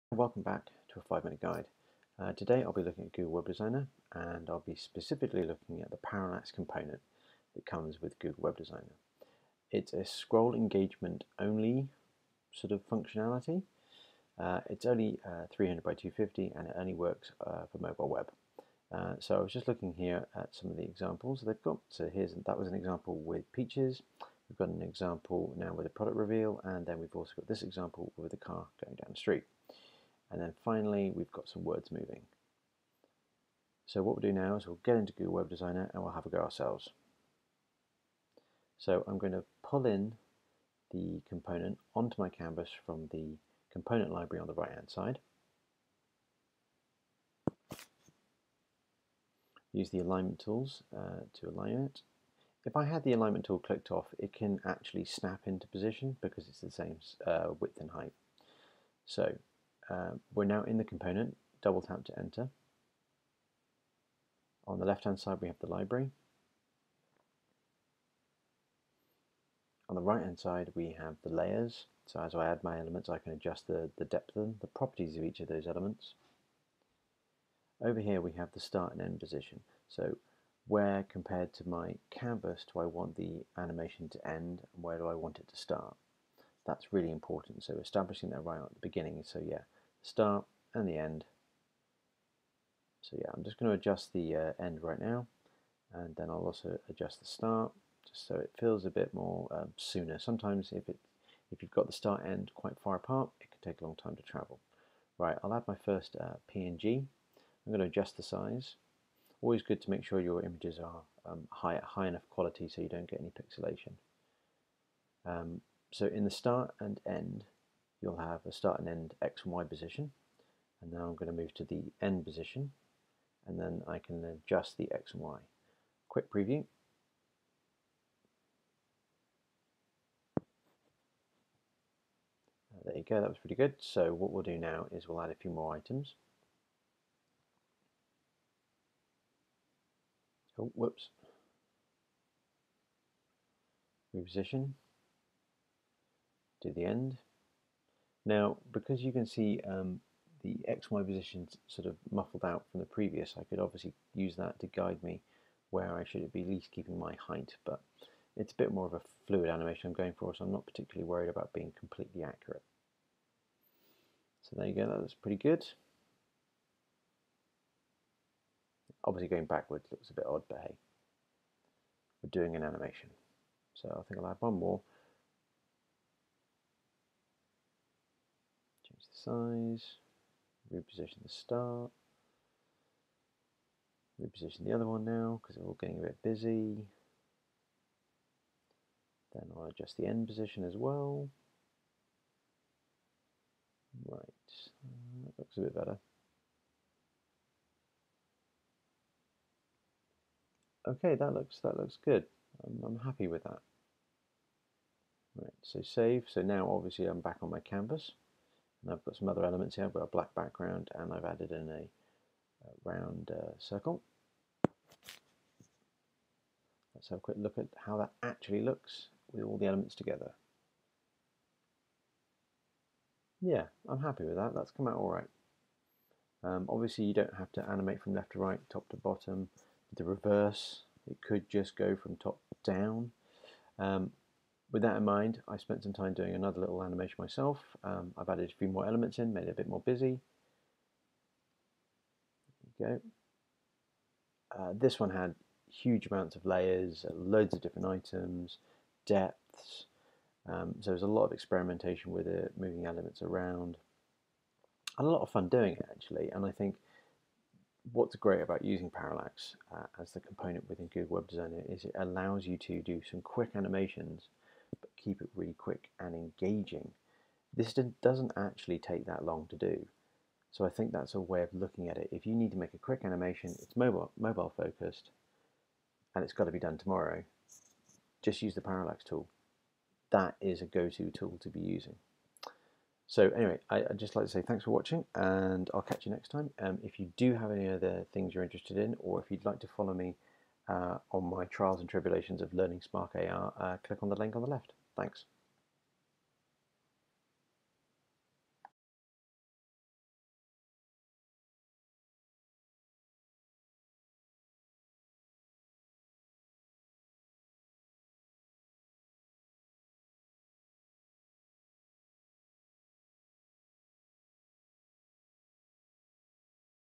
Google Web Designer Parallax Ad Component guide
This is a 5-minute Tutorial animating with the parallax component available in Google Webdesigner.
Sorry, the audio is a little low.